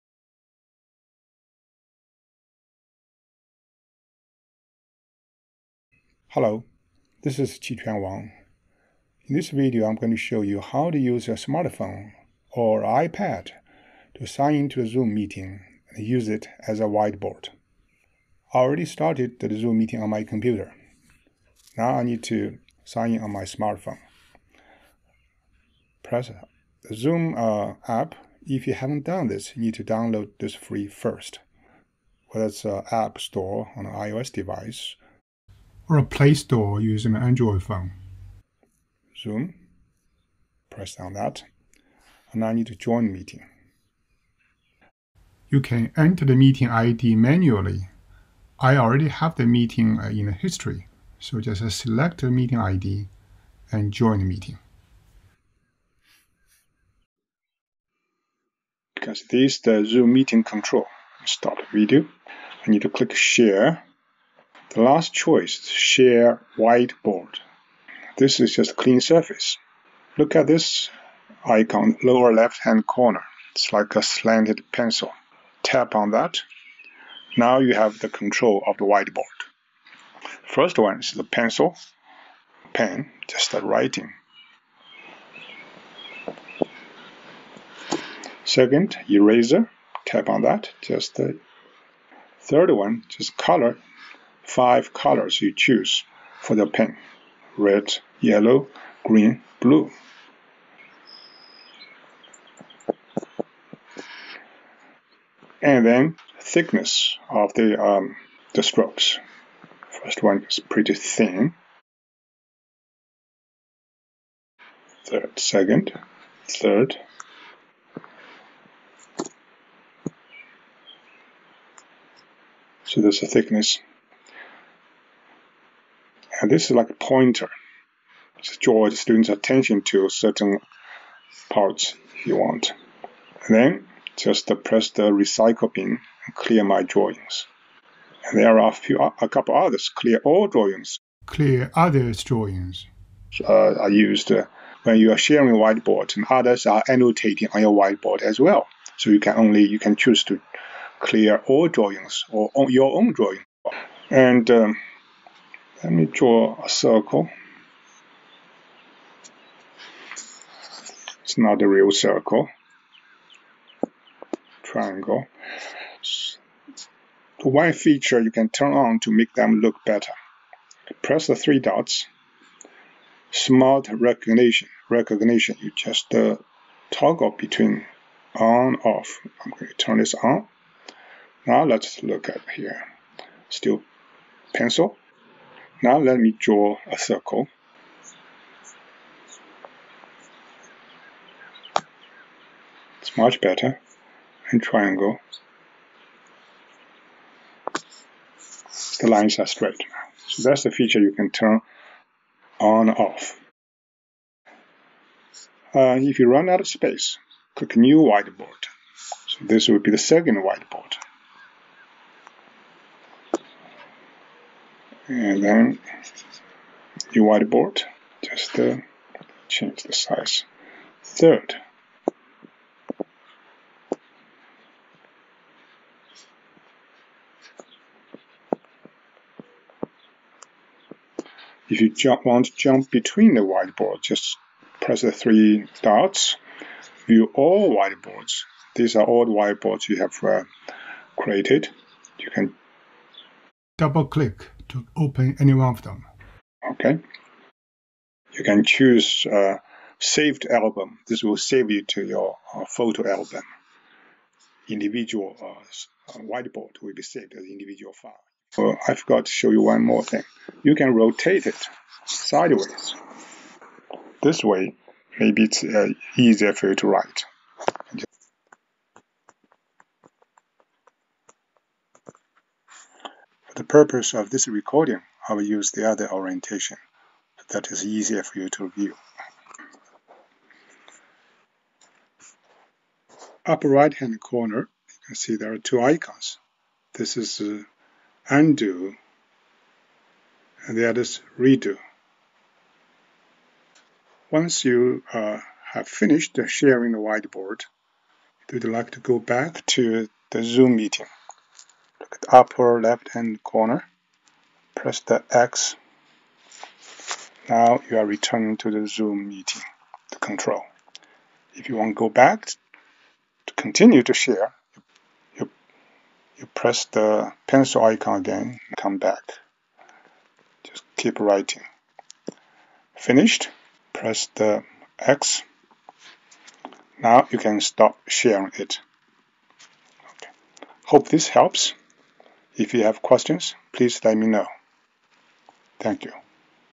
Using a Smart Phone as a Whiteboard
0:00 -Joining a Zoom Meeting Using a Smartphone

1:01 -Share Whiteboard

1:20 -Whiteboard Tools

3:21 -Using Multiple Whiteboards

4:08 -Saving Whiteboards

4:41 -Undo/Redo Whiteboard Edits

4:54 -Returning to Zoom Meeting

This video demonstrates how to use a Smart phone as a Zoom Whiteboard. An Android phone is used in this demo. A tablet or an iPhone would work similarly.